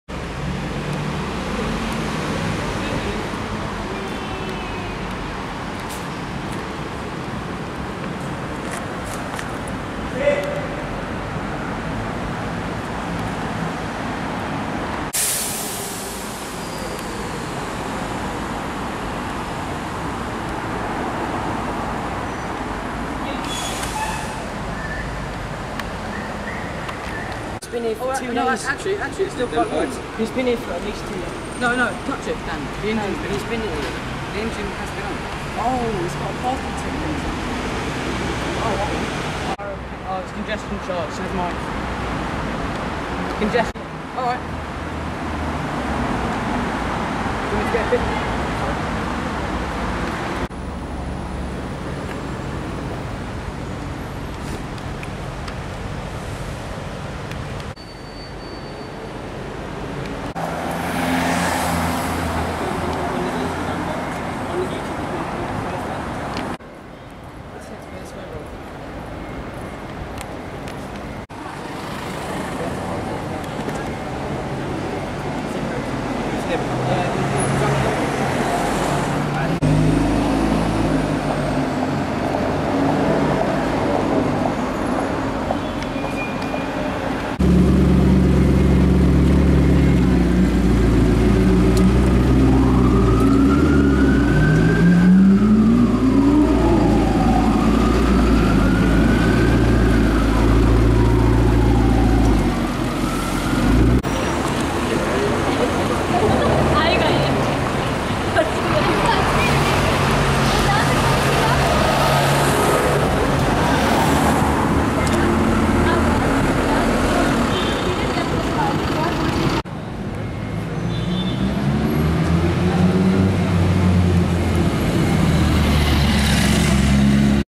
Shmee150's Cars: McLaren 12C | Morgan 3-Wheeler | Abarth 595C Turismo
Hey everyone, whilst filming cars in London i've filmed Tim Burton's (Shmee150) cars parked and driving around. He has both his McLaren 12C and Morgan 3-Wheeler wrapped in a very eye catching shade of blue by 'Dub Customs'.